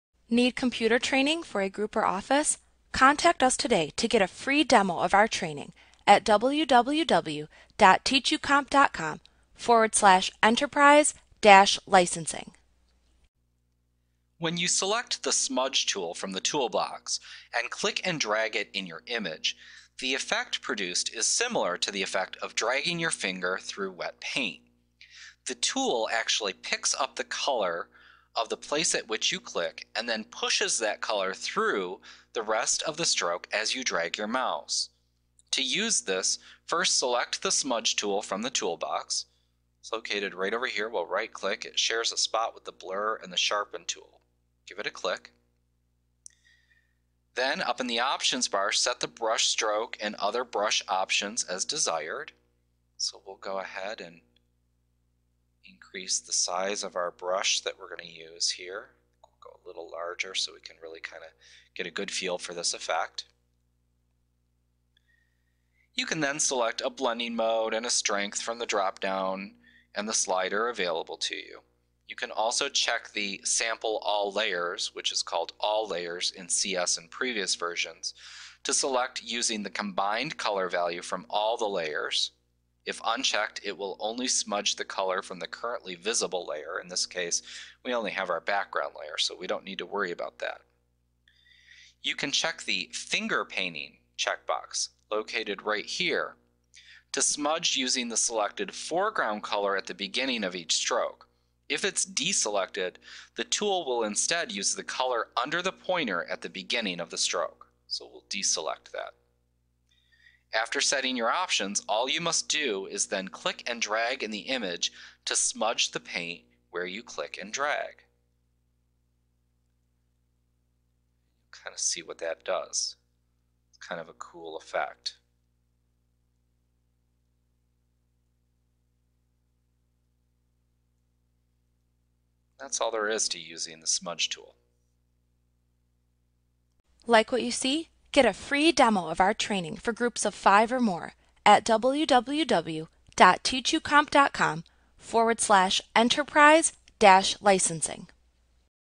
Photoshop CS6 Tutorial The Smudge Tool Adobe Training
FREE Course! Learn about the smudge tool in Adobe Photoshop A clip from Mastering Photoshop Made Easy v. CS6. - the most comprehensive Photoshop tutorial available. Visit us today!